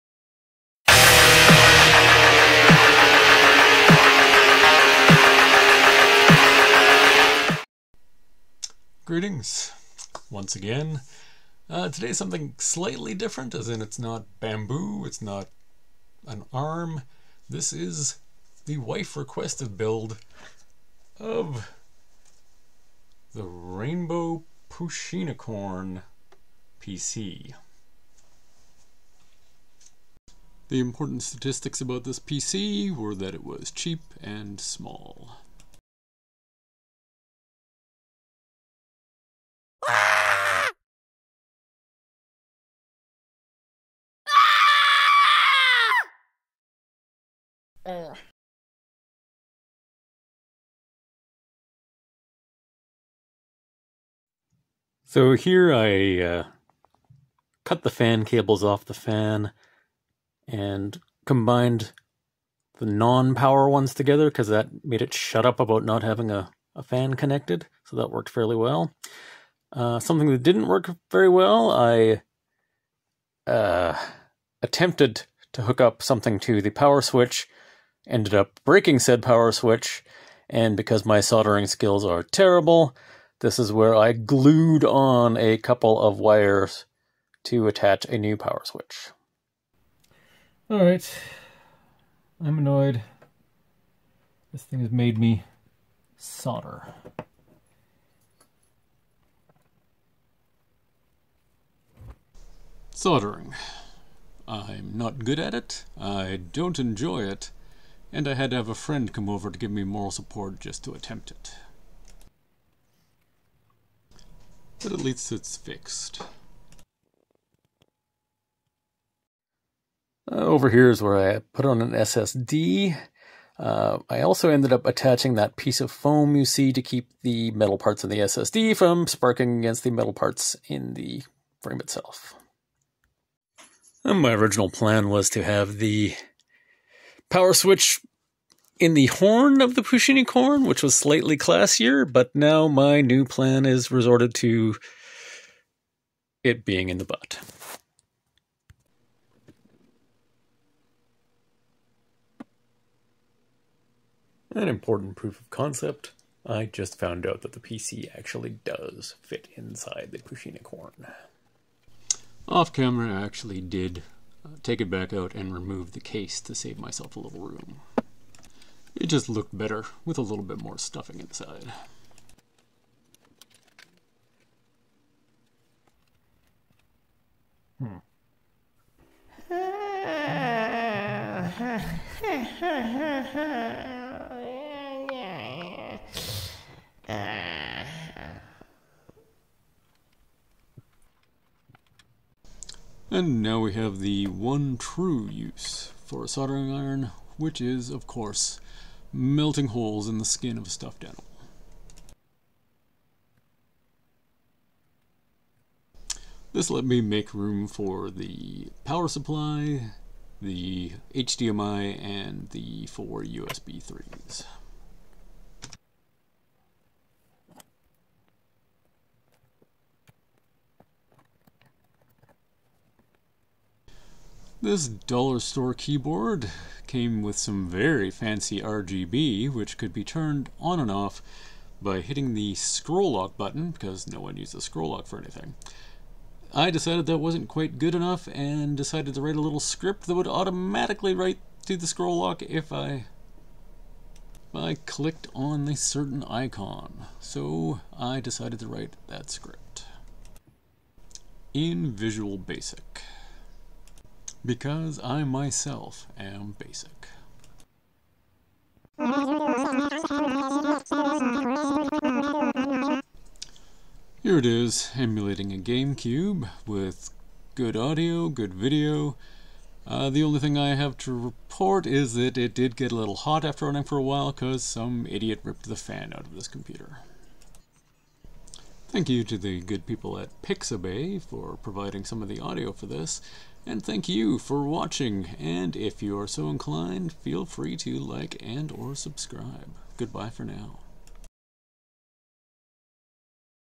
Rainbow Pusheenicorn PC
Mounting a full mini PC in the body of a stuffed toy, specifically the Rainbow Pusheenicorn.